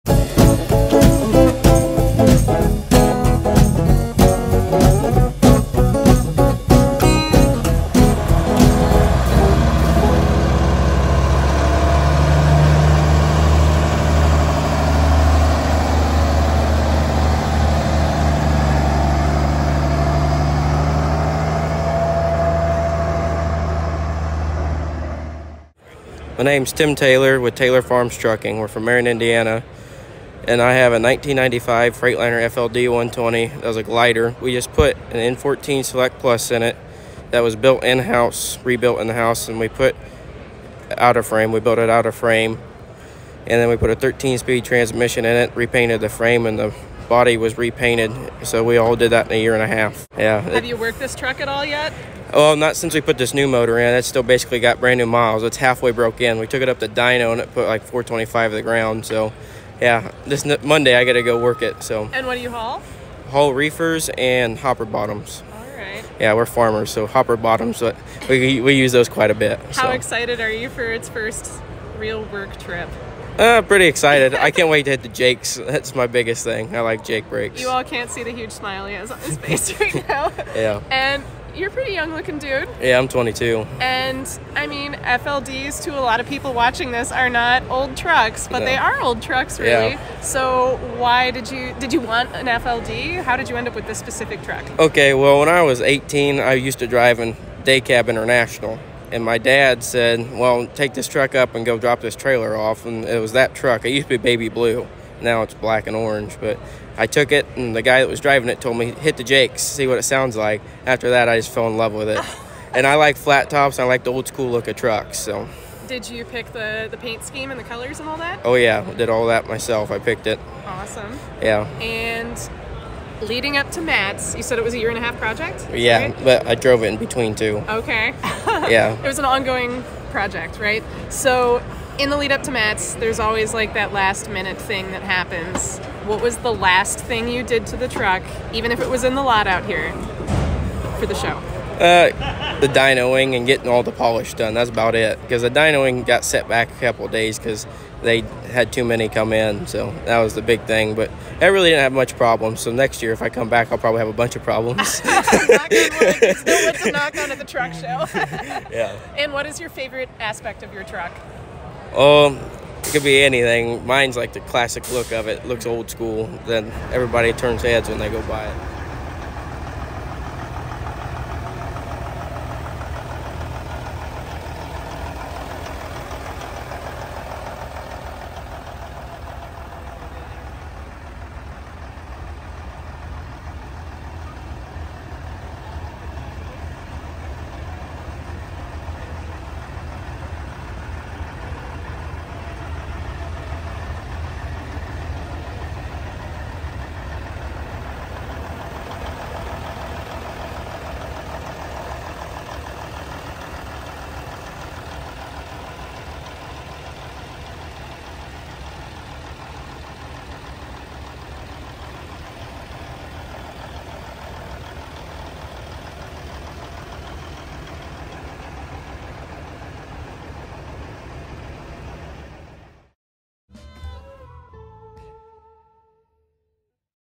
Tim Taylor’s 1995 Freightliner Truck Tour
Filmed at the 2023 mid-America Trucking Show in Louisville, KY.

Miss Flatbed Red is an artist who specializes in trucks.